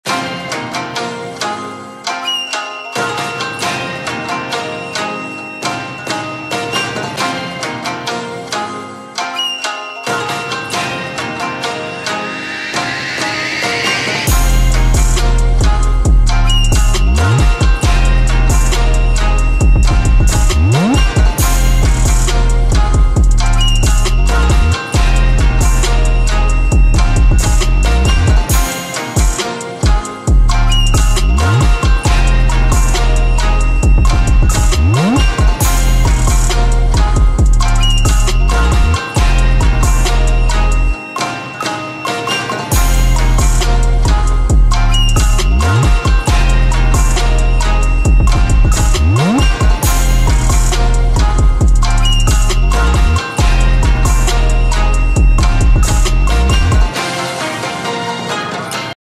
EGYPT - Cairo, Giza Pyramids and Luxor | WATCH THIS!
A glimpse of my trip to Cairo, Giza Pyramids, Luxor, Karnack Temple and Valley of the Kings.